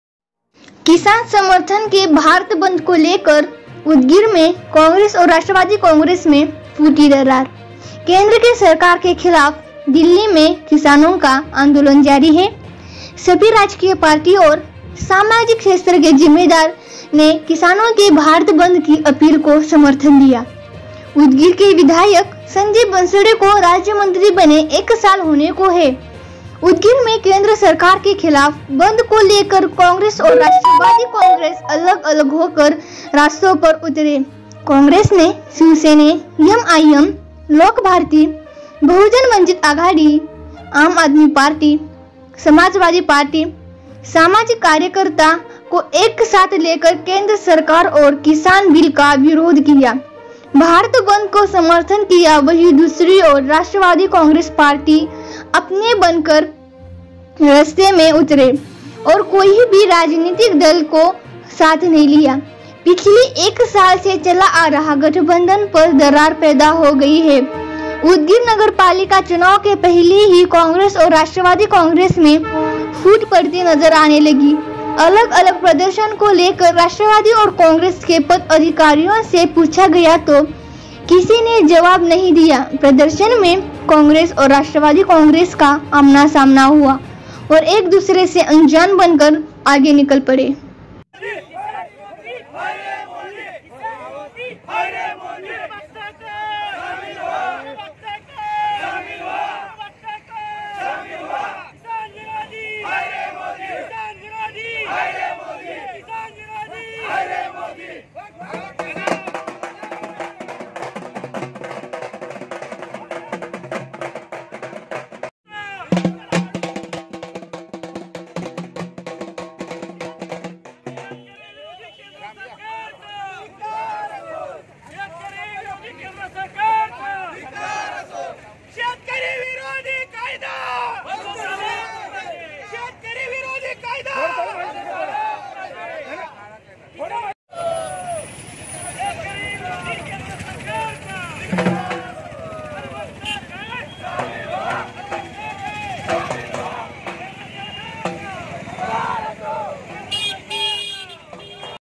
केंद्र सरकार के खिलाफ़ उदगीर में आंदोलन
किसान समर्थन के भारत बंद को लेकर उदगीर में कांग्रेस और राष्ट्रवादी कांग्रेस में फूटी दरार